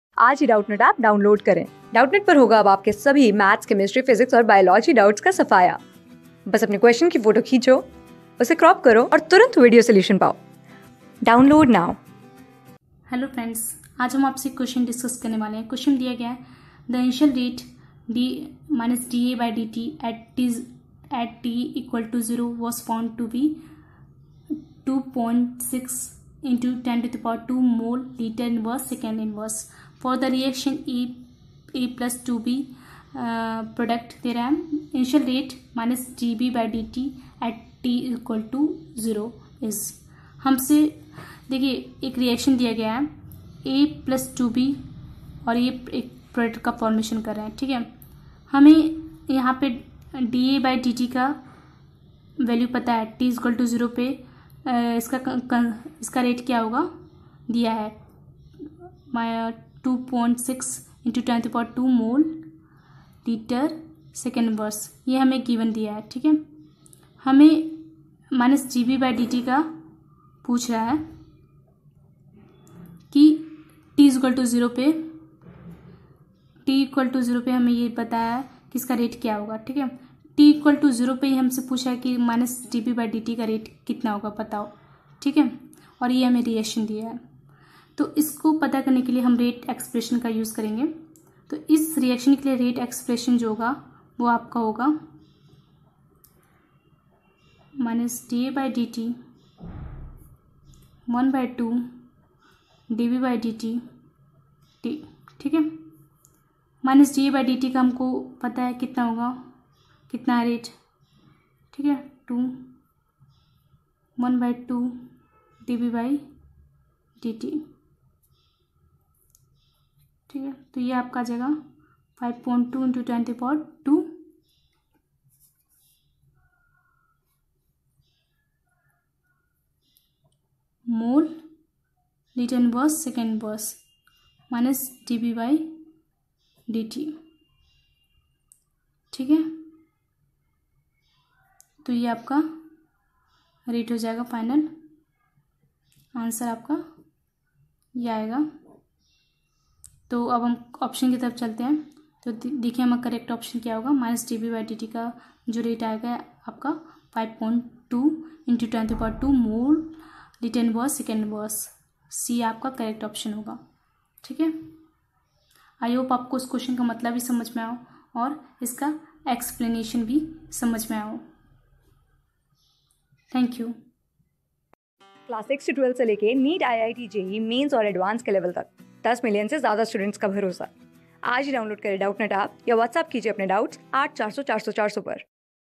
The initial rate, -(d[A])/(dt) at t=o was found to be 2.6xx10^(2) mol L^(-1)s^(_1) for the react...
The initial rate, -(d[A])/(dt) at t=o was found to be 2.6xx10^(2) mol L^(-1)s^(_1) for the reaction A+2Brarrproducts The initial rate, -(d[B])/(dt), at t=o is
Class: 12
Subject: CHEMISTRY
Chapter: CHEMICAL KINETICS
Board:Maharashtra Board

👉 Have Any Query? Ask Us.